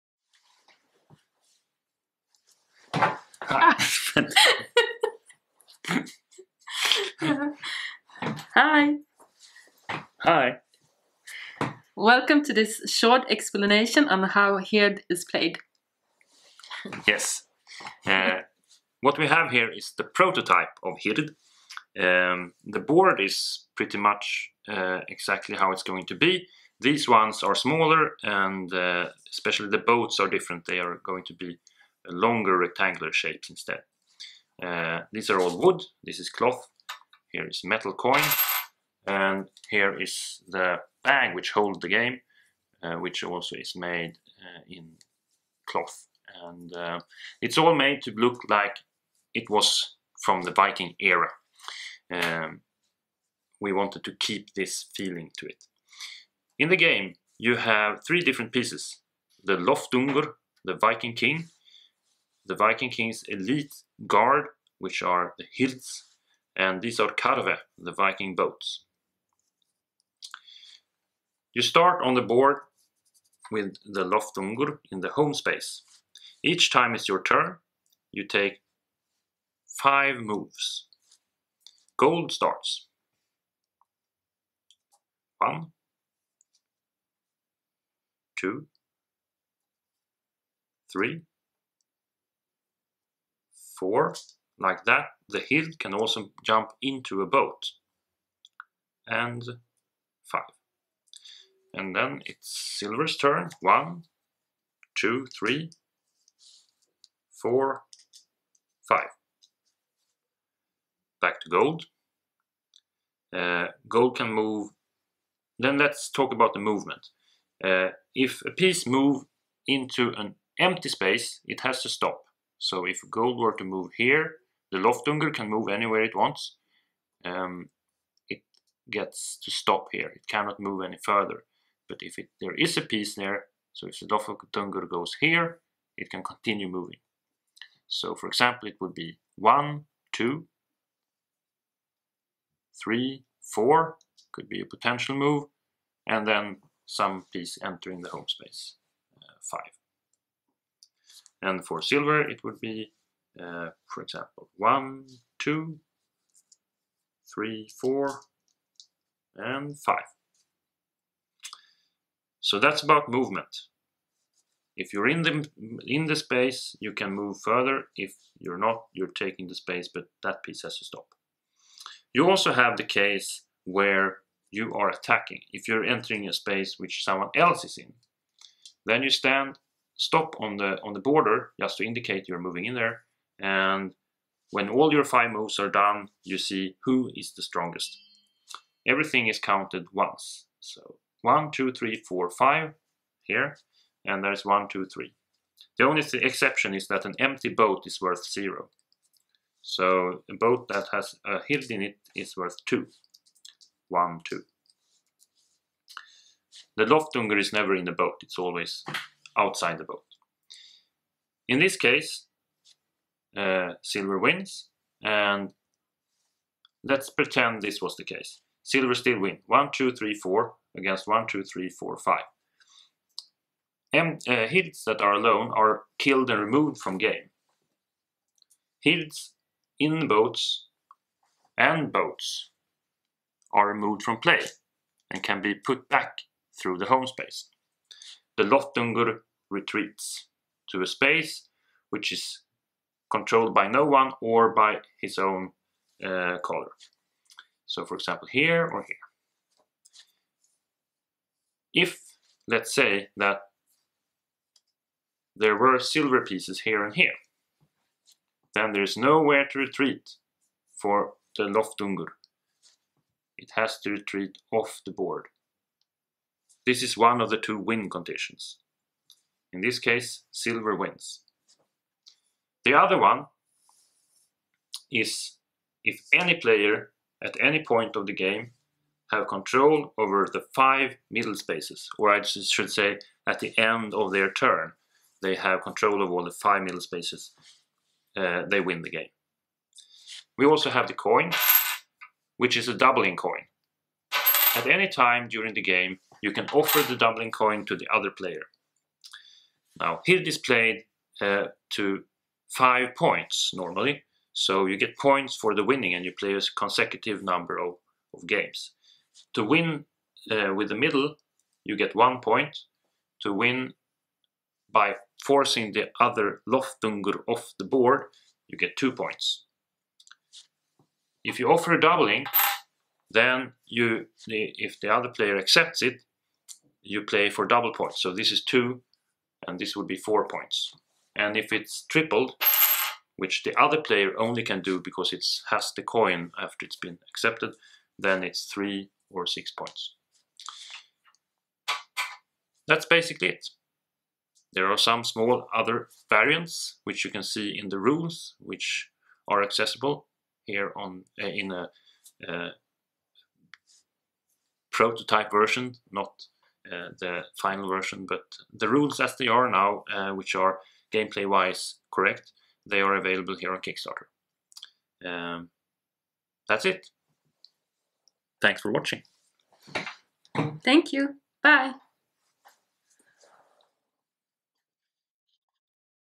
Game play
Challenge your friends in this ingenious and portable game, uniquely crafted to give you the feeling of Viking era gaming. HIRÞ is a 2-player strategy game that is designed to look, feel and play like a game from the Viking times.The gameplay of HIRÞ may look simple at first, at least for an experienced gamer. But in playtesting, many players have commented on the unexpected depth of the strategies and how new potential tactics kept unfolding. Here is a quick overview of the gameplay.